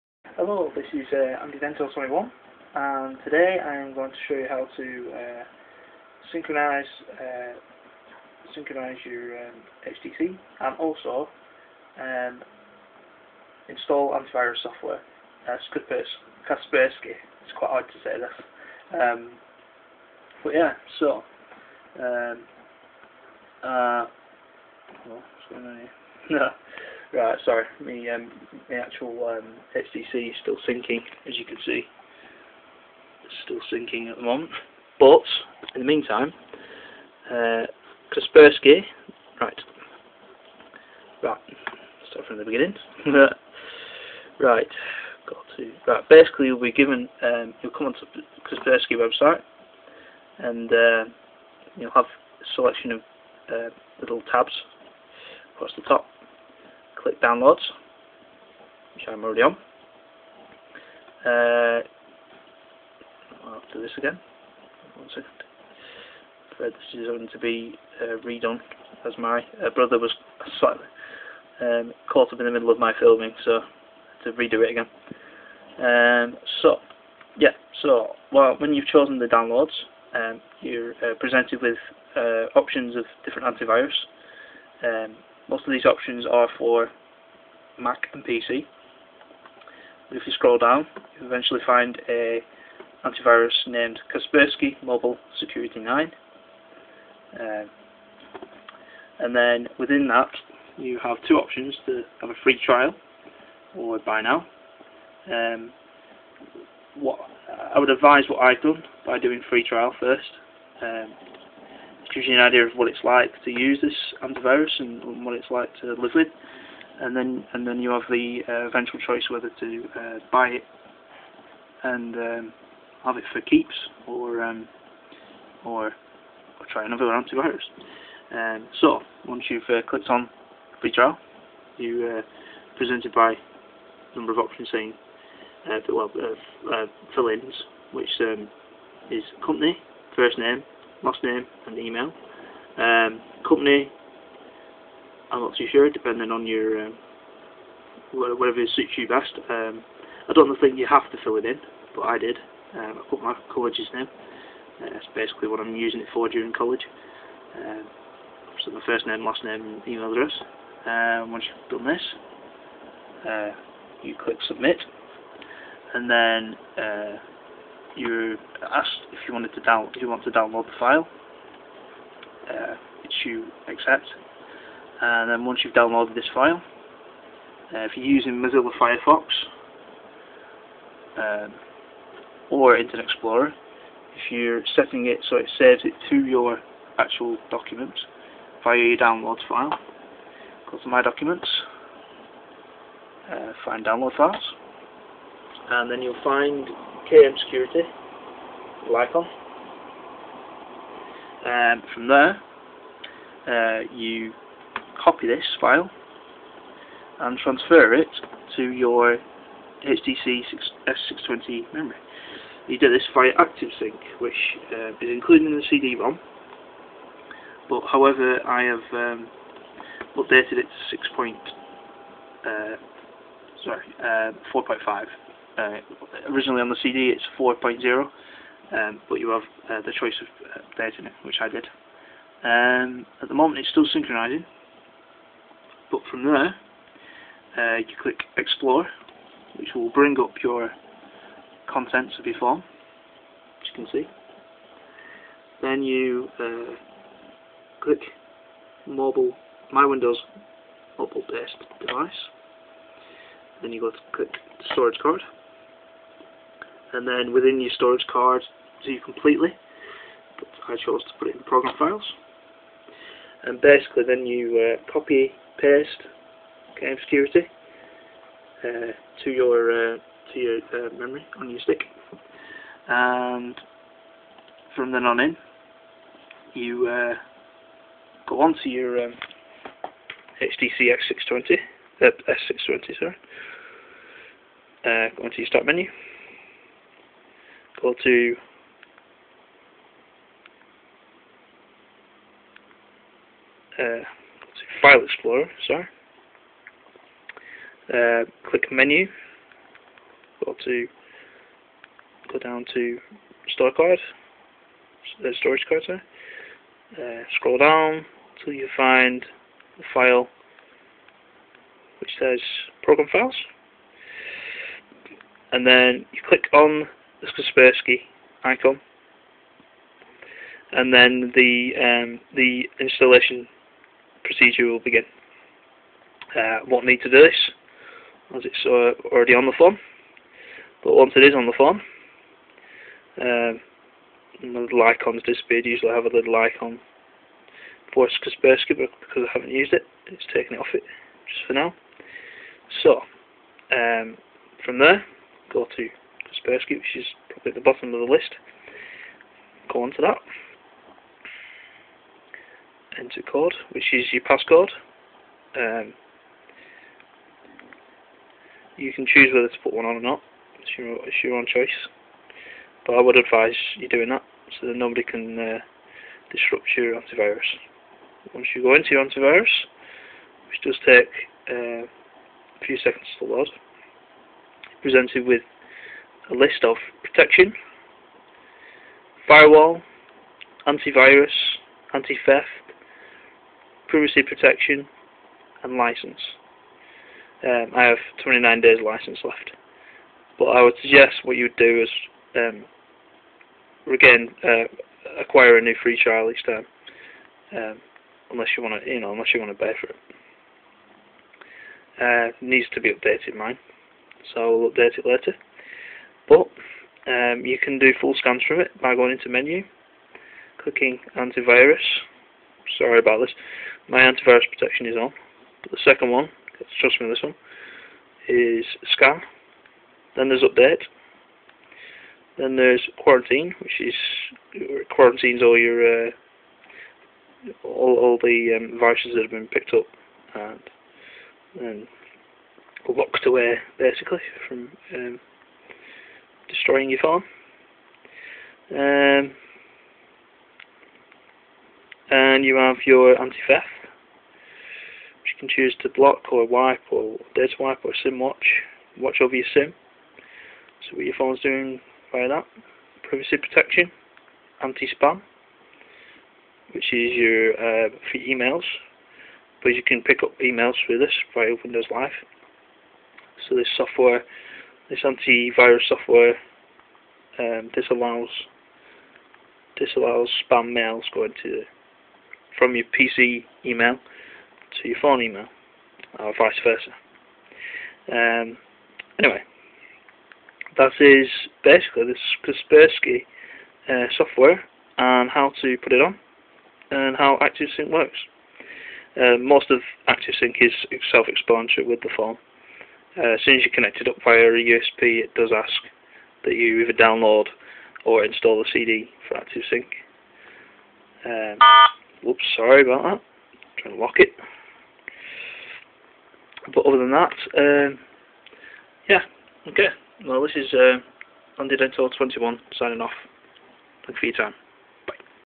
Kaspersky Mobile Anti Virus Software (Free Trial)
This is a video on how i installed this mobile anti virus software onto my HTC S620